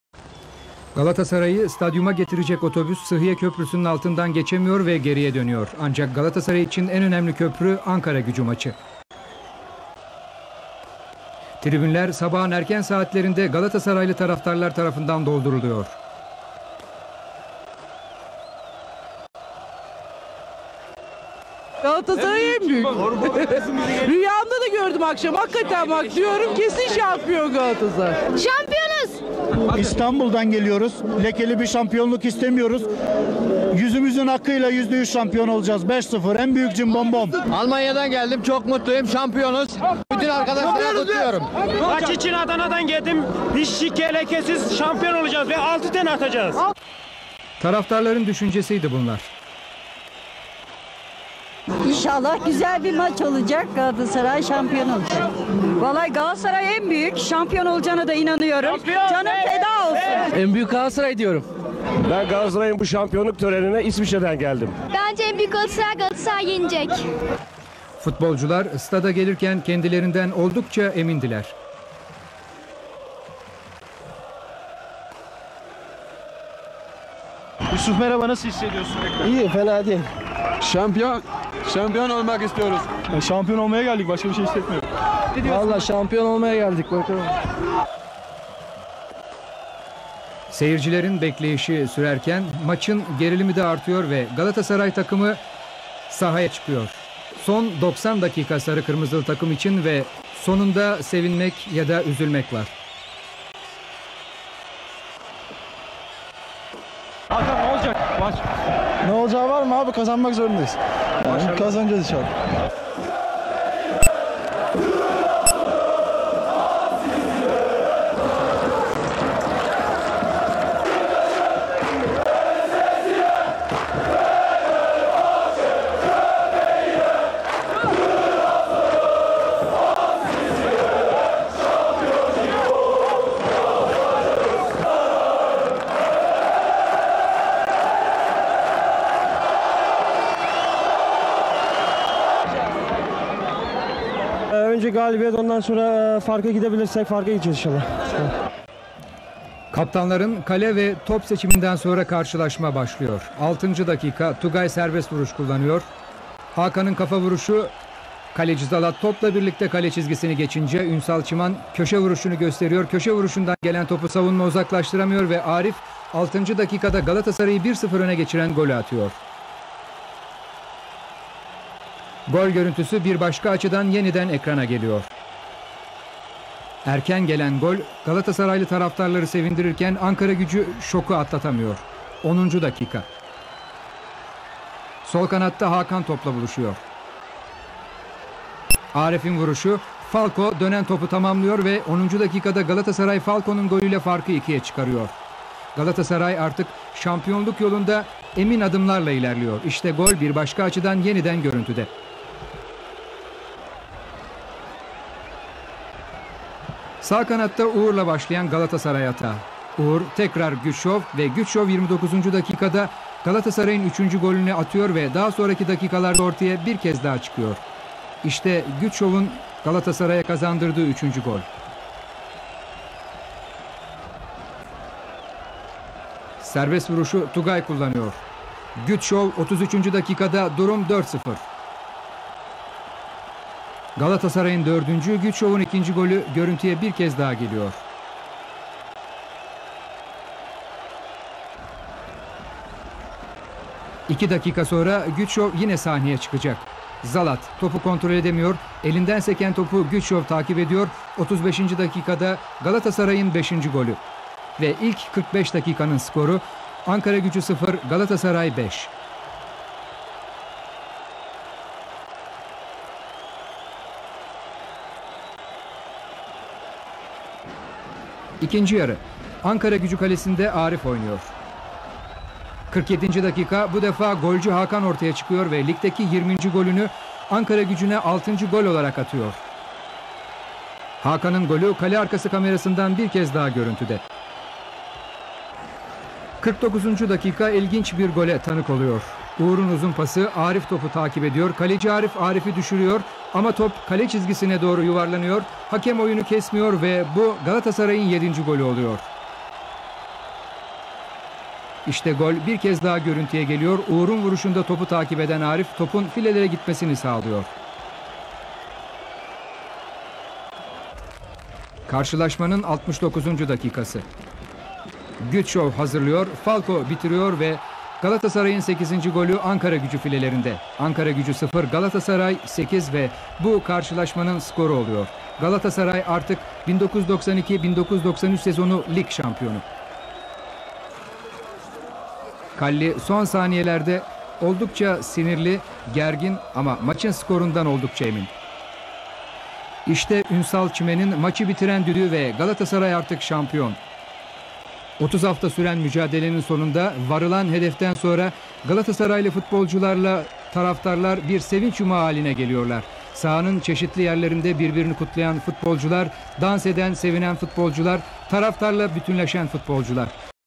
Ankaragücü 0-8 Galatasaray 30.05.1993 - 1992-1993 1st League Matchday 30 Before-Match Comments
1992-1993 Sezonu 1. Lig 30. Hafta Maçı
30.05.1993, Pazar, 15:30
Ankara 19 Mayıs
Ankara, Türkiye